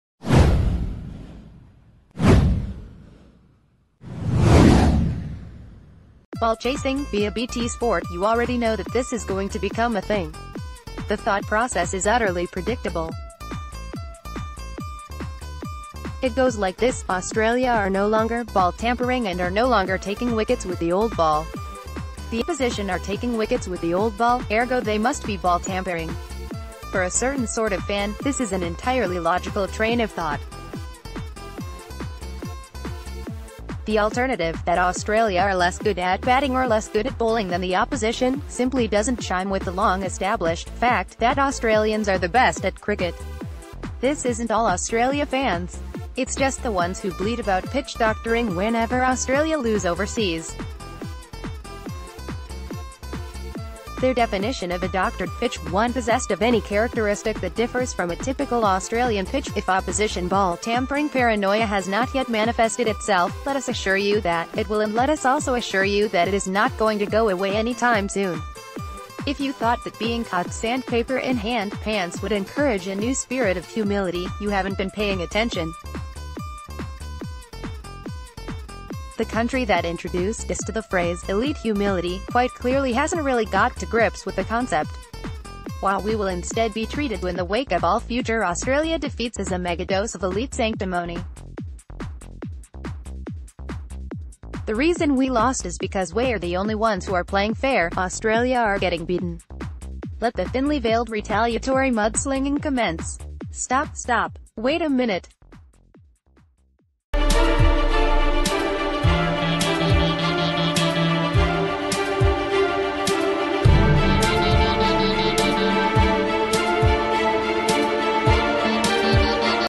Have Australia fans become paranoid about opposition ball tampering yet?
Ball-chasing (via BT Sport) You already know that this is going to become a thing. The thought process is utterly predictable. It goes like this: Australia are no longer ball tampering and are no longer taking wickets with the old ball. The opposition are taking wickets with the old ball, ergo they must be ball-tampering. For a certain sort of fan, this is an entirely logical train of thought. The alternative – that Australia are less good at batting or less good at bowling than the opposition –...